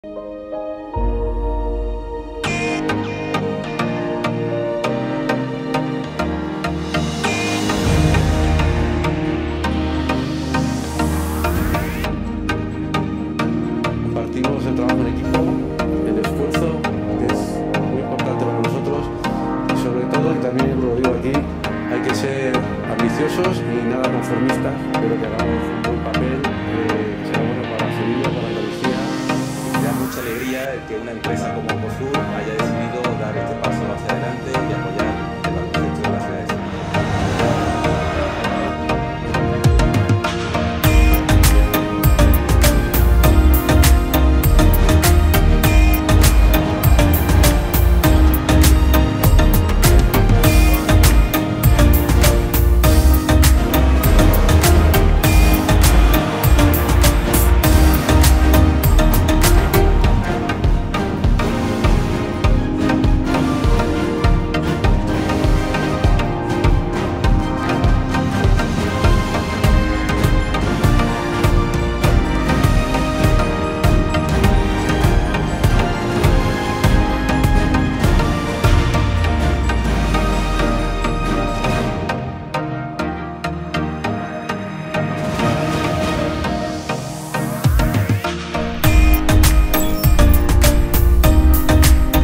Coosur y el Real Betis renuevan su colaboración para las temporadas 2020/2021 y 2021/2022
El equipo de baloncesto de Sevilla seguirá denominándose Coosur Real Betis durante las dos próximas temporadas. En este vídeo podéis ver algunas de las imágenes más destacadas de los momentos que vivimos la pasada temporada.